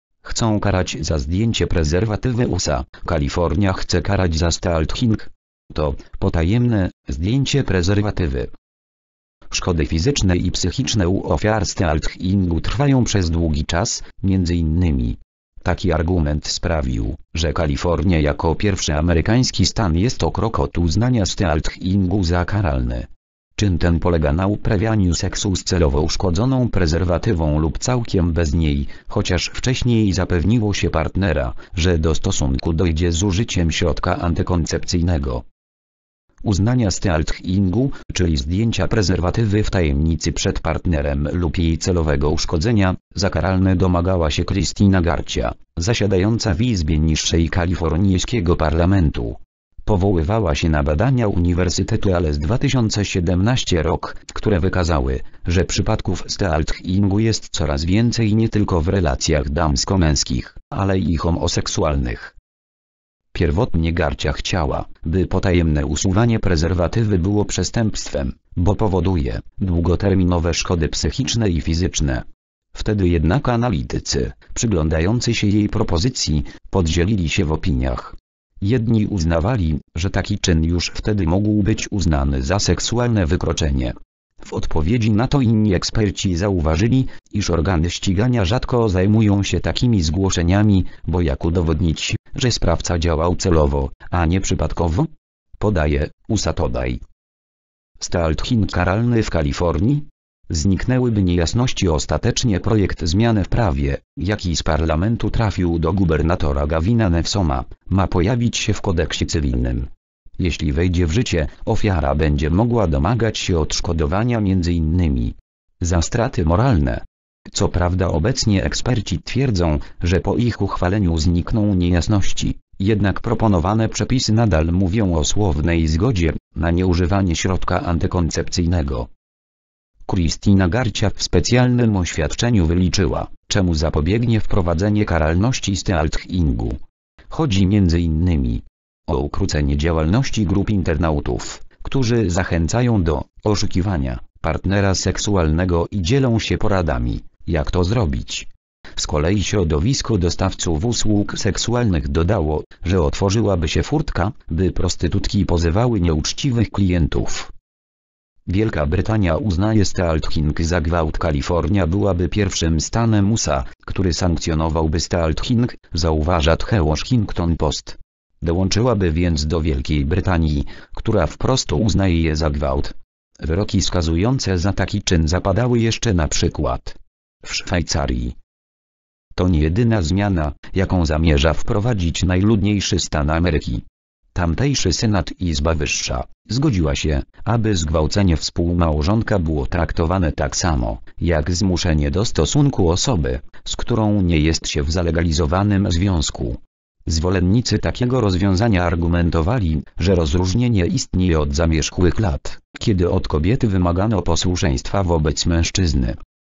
Chcą karać za zdjęcie prezerwatywy.
USA: Kalifornia chce karać za stealthing. To "potajemne" zdjęcie prezerwatywy.

  (fot. 123RF/PICSEL/RATTANAKUN THONGBUN)